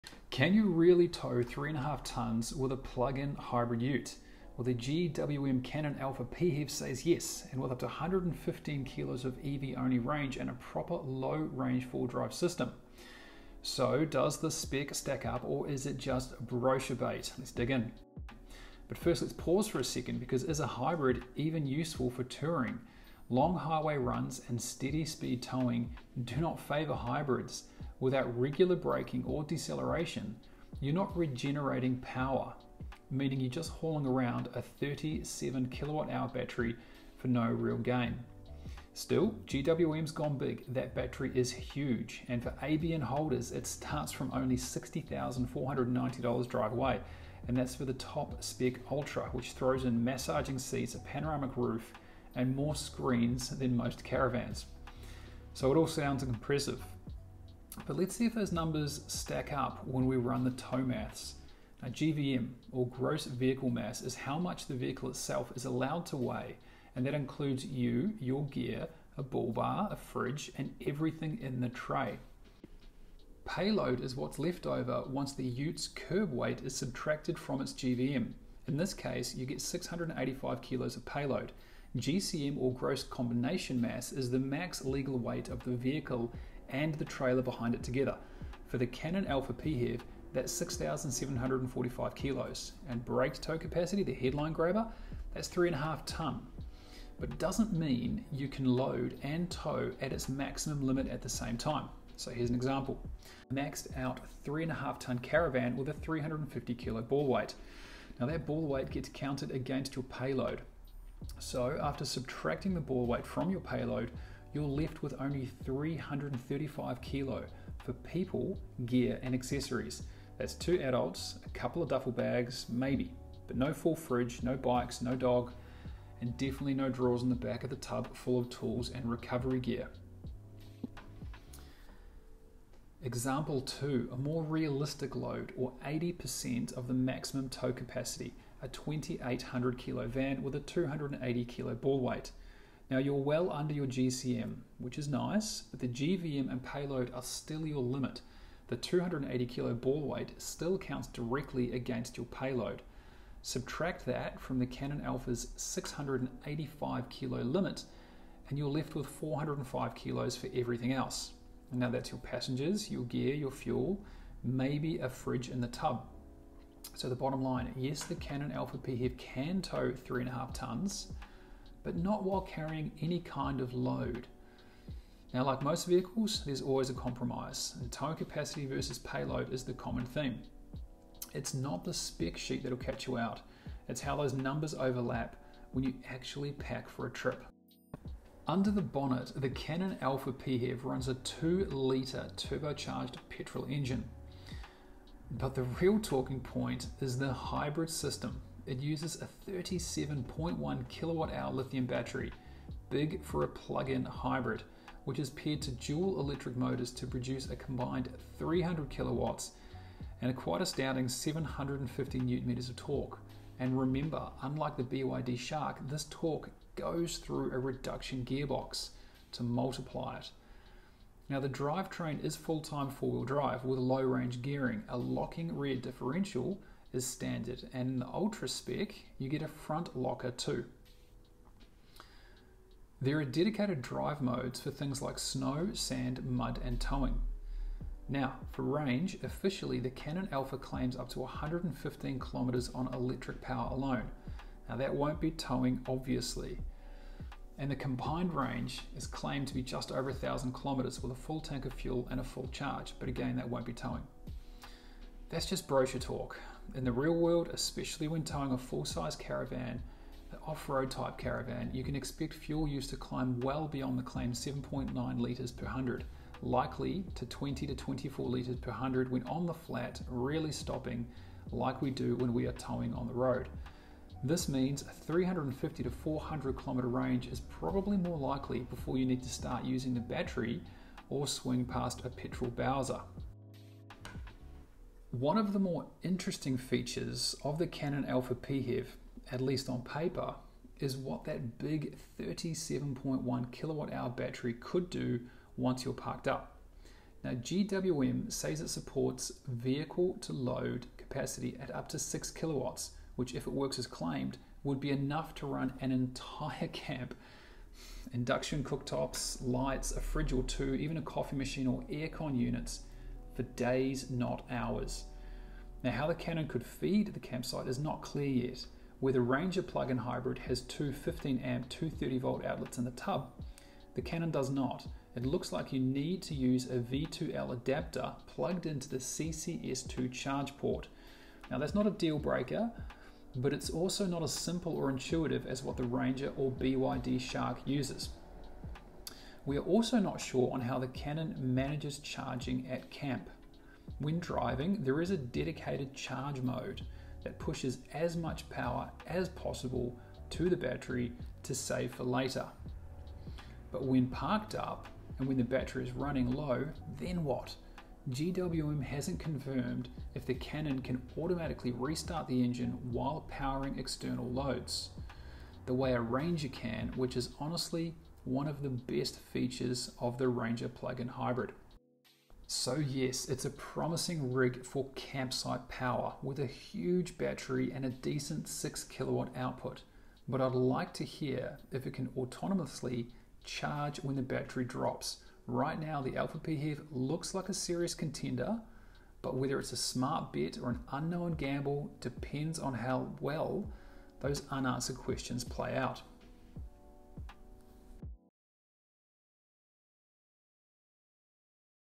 Tow Check: GWM Cannon Alpha PHEV
Can you really tow 3.5 tonnes with a plug-in hybrid ute?

In this episode of Tow Check, Tim breaks down the GWM Cannon Alpha PHEV Ultra, Australia's most ambitious hybrid dual-cab. With 750Nm of torque, a 37.1kWh battery, and claims of 6kW campsite power output, it looks great on paper — but is it ready for the real world?

We put the specs under the microscope:

Payload, GVM and GCM maths
Towing scenarios with a 3,500kg and 2,800kg caravan
Real-world fuel use vs brochure range
Vehicle-to-load (V2L) power and campsite charging concerns
How it stacks up against the Ford Ranger PHEV and BYD Shark

This isn't a test drive — it's a desk-shot deep dive into whether the Cannon Alpha PHEV is ready to work, or still has something to prove.

🔧 Full tow maths breakdown
🔋 V2L vs real camp power
📊 Payload limits that might surprise you